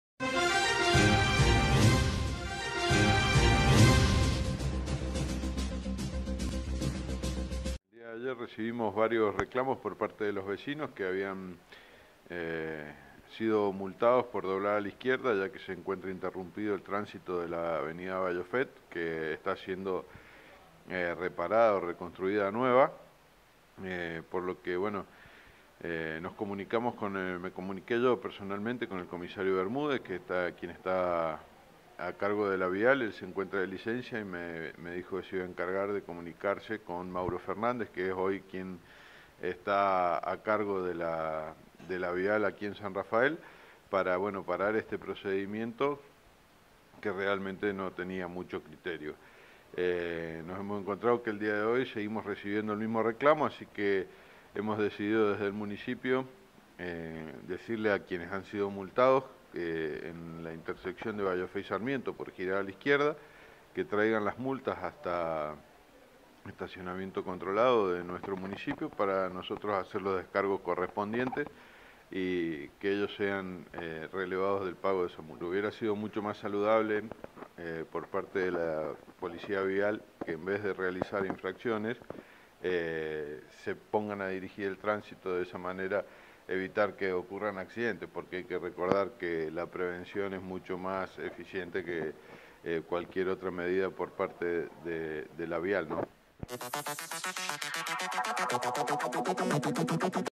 DESPRESTIGIAR LA POLÍTICA
Varios vecinos del departamento fueron multados, luego de girar a la izquierda en la intersección de las avenidas Ballofet y Sarmiento. Desde el municipio, se inició un proceso formal para que fuesen desestimadas.